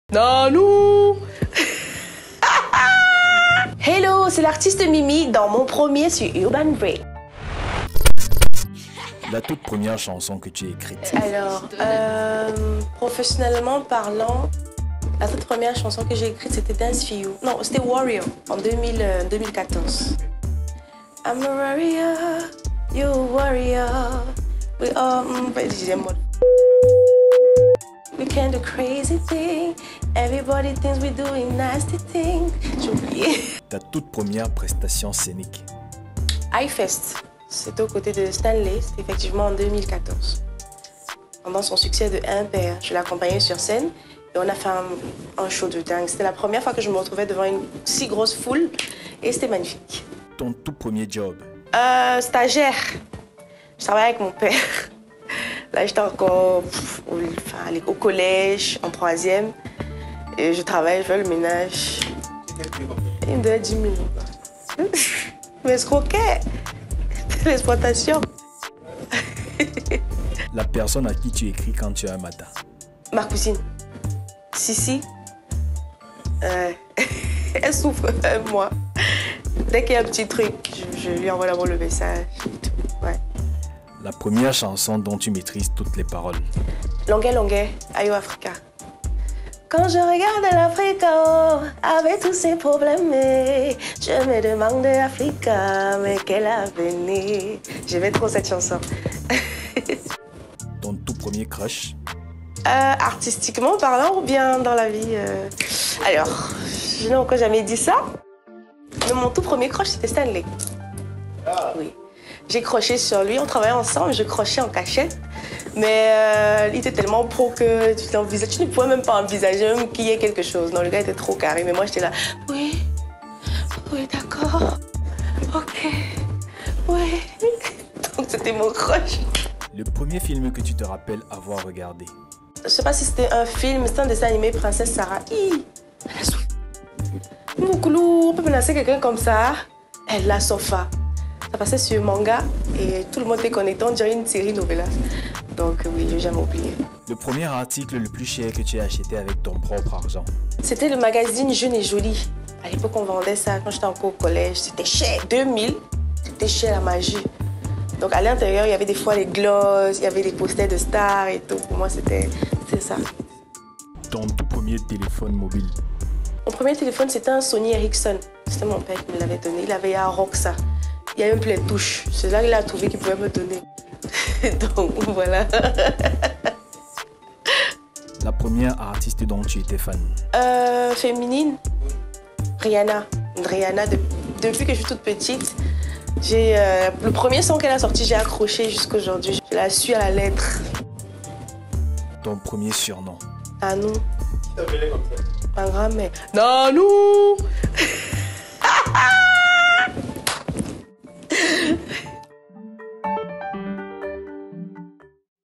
MON PREMIER || MIMIE 🔵🔵🔵
Saviez-vous que son premier Surnom c'était "Nanou"? 😅😅
Saviez-vous que son premier crush c'était @StanleyEnow  😍😍😍
Nous avons reçu @mimieofficial185  dans notre émission MON PREMIER by UrbanBridge ❤️💋💯💙🔵🔵